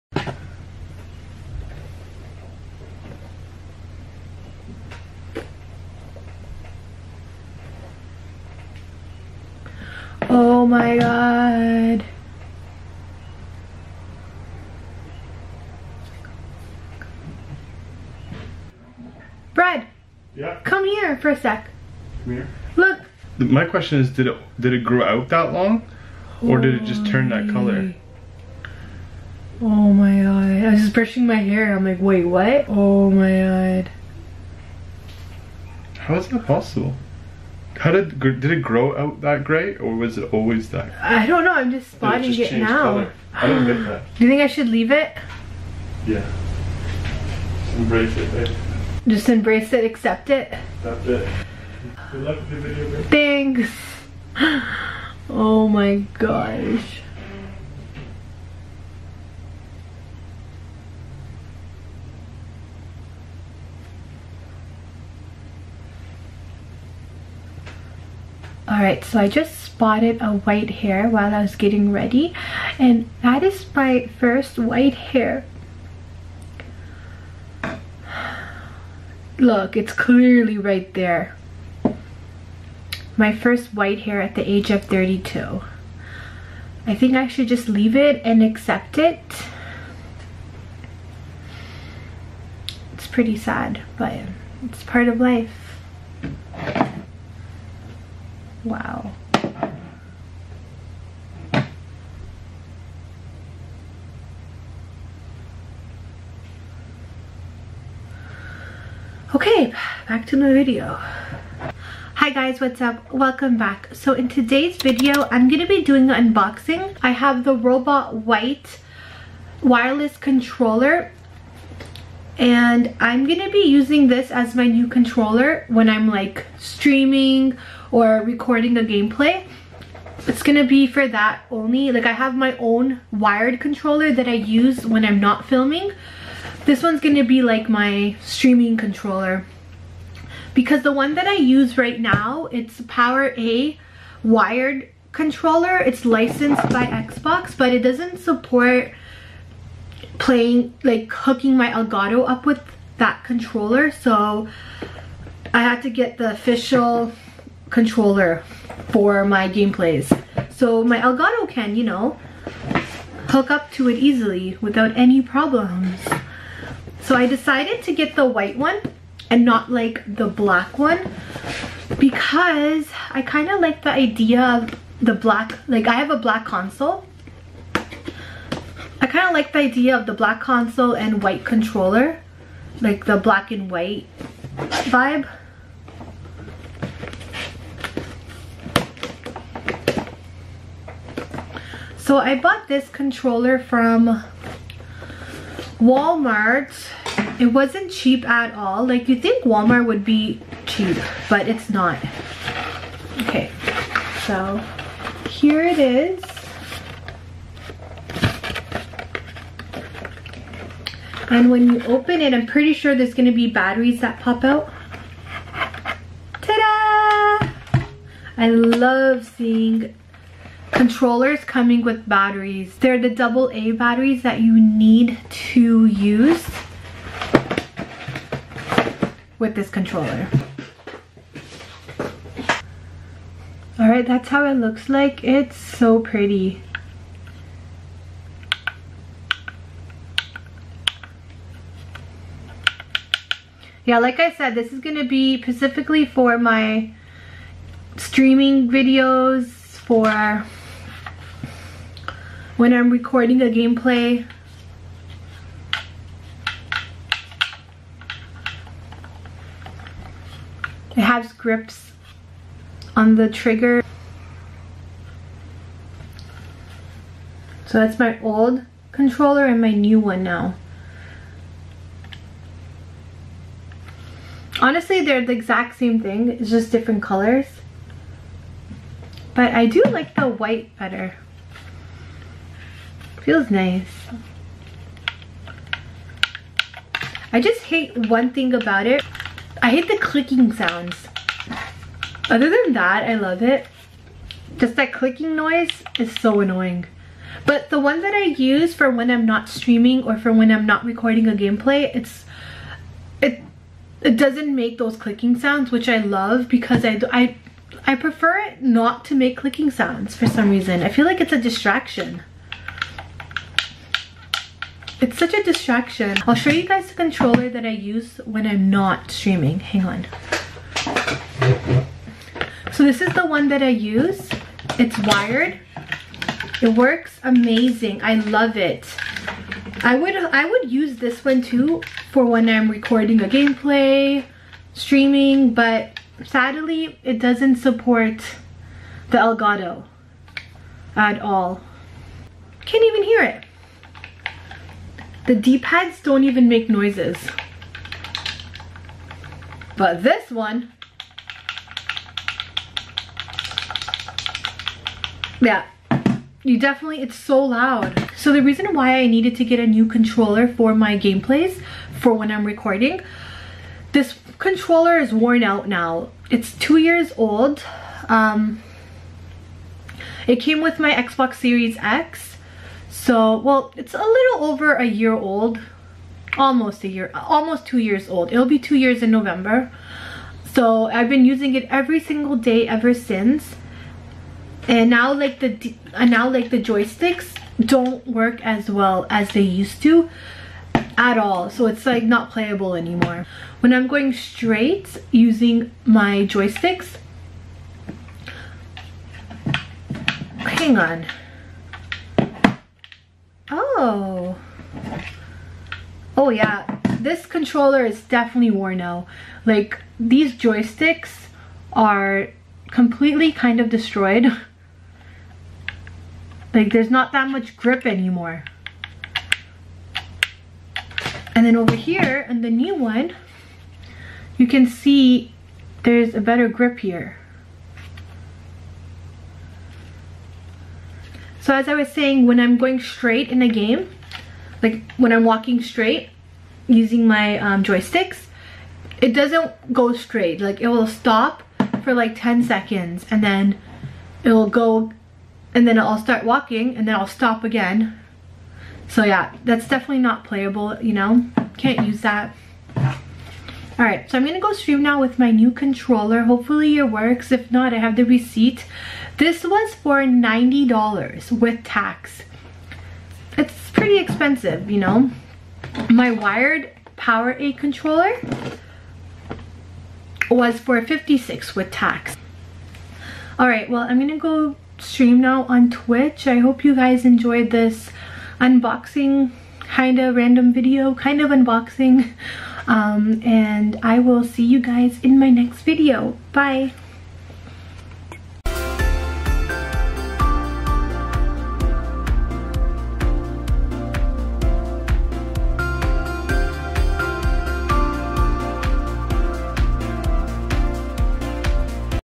Unboxing Robot White Xbox controller 2022. (My 1st white hair caught on camera)
Follow for updates.